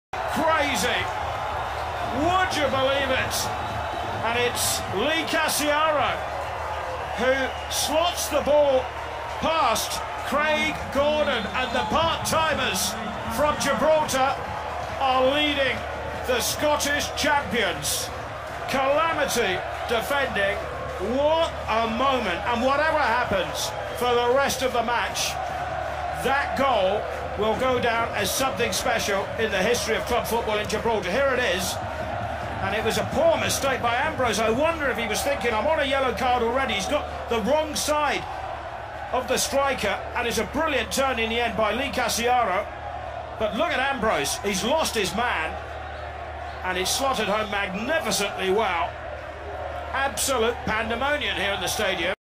Lee Casciaro Goal for lincoln fc win against Celtic !!!! 1-0
Lee Casciaro Goal  enjoy this win lincoln fc